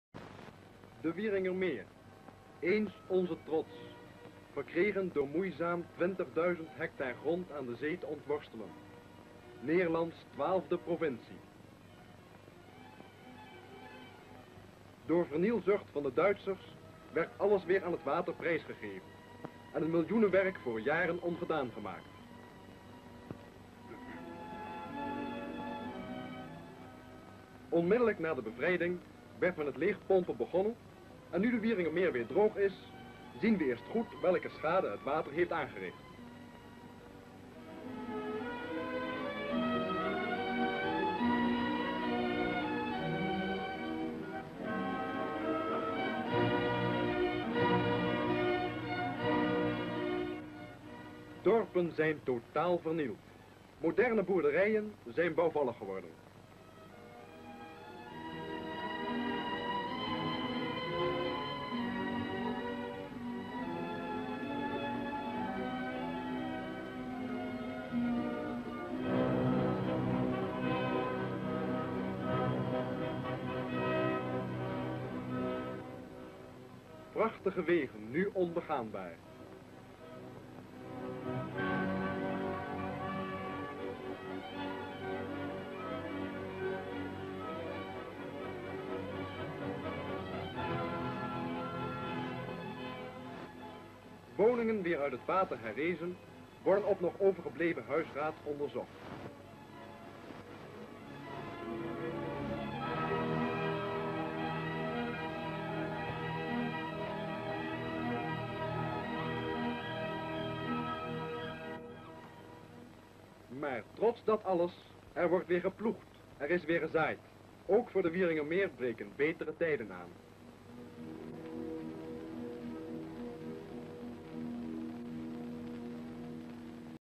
Wieringermeer dry again (Weeknummer 45-51) 🔴
Description (more): Atmosphere shots of the situation in the Wieringermeer before the second World War: city areas, machine harvesting of grain and hay pile up on a boerenkar. Shots shot during a sailing trip through the flooded polder. Shots of pipelines through which the water is pumped from the polder; shots of the barren polder; shots of the damage done: houses, farms and barns have been completely destroyed; the ground is covered with a layer of mud; roads are impassable; the population works to repair the damage; a farmer plows again the country.